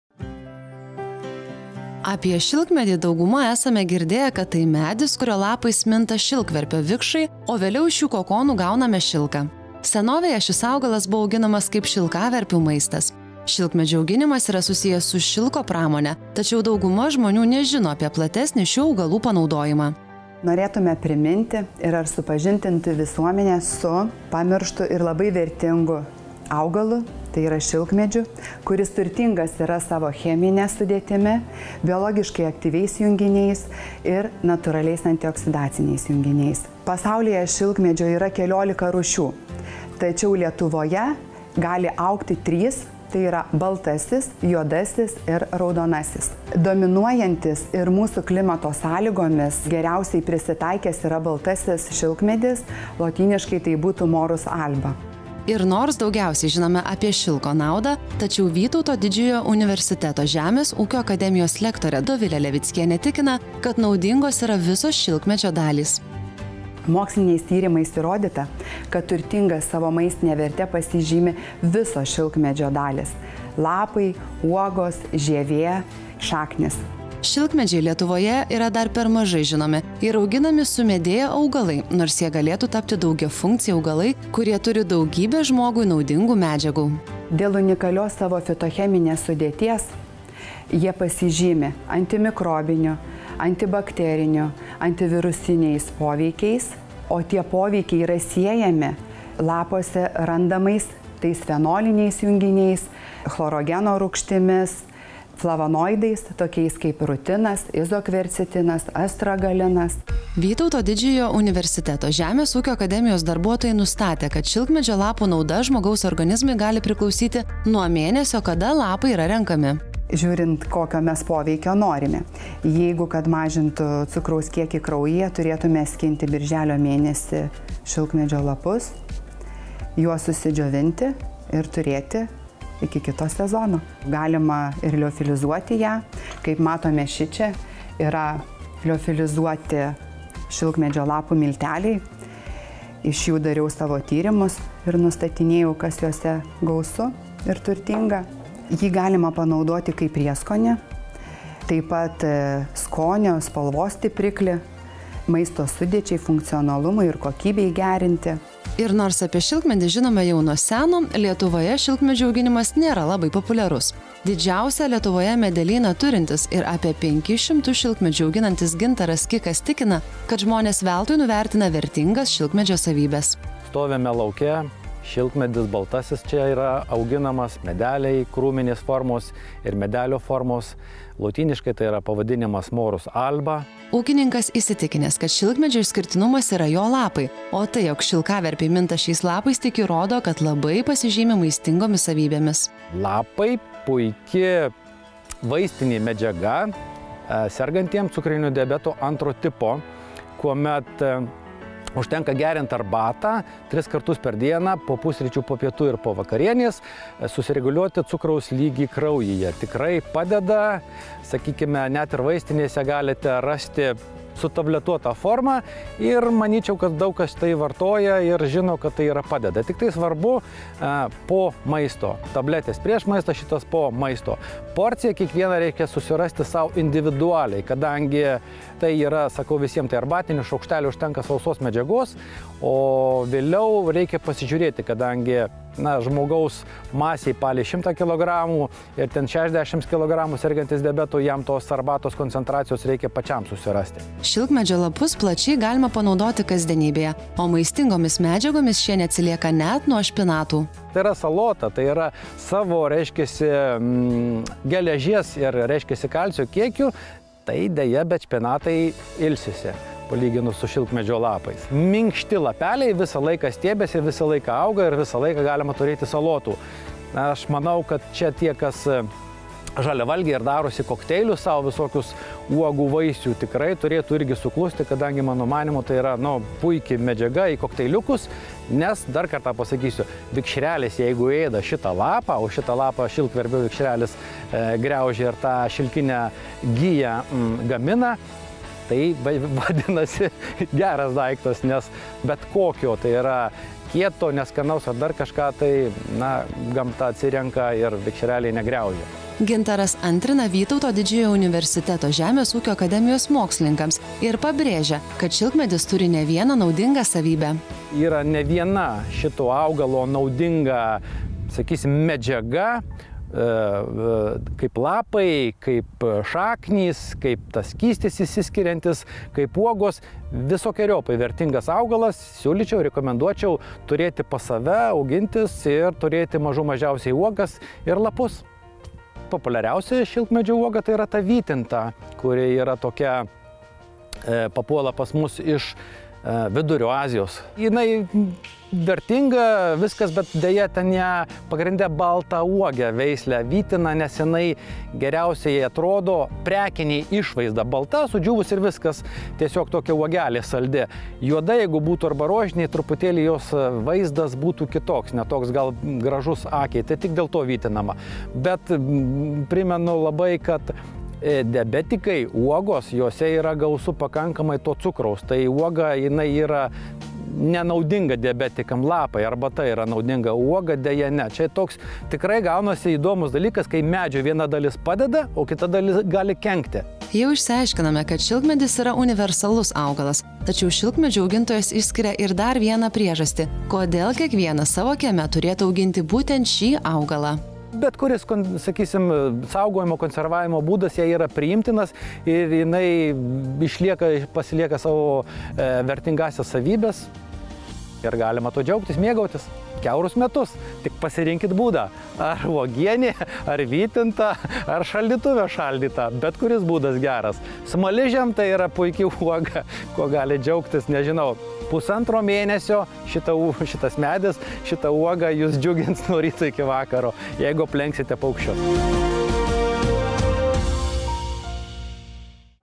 SVEIKATOS MEDIS - šilkmedžio lapai geležimi ir kalciu lenkia net špinatus
Šilkmedis – tai medis, kurio lapais minta šilkverpio vikšrai. Senovėje šis augalas buvo auginamas kaip šilkaverpių maistas, tačiau šiandien dėl turtingų savo maistinių savybių, vis dažniau įtraukiamas į receptus, naudojamas maisto papildų gamyboje ar liaudies medicinoje. Moksliniais tyrimais įrodyta, jog naudingos yra visos šilkmedžio dalys: lapai, šaknys, uogos, žievė, o kasdien geriama šilkmedžio lapų arbata yra puiki priemonė diabetikams, siekiant sureguliuoti cukraus kiekį kraujyje. (S6E1)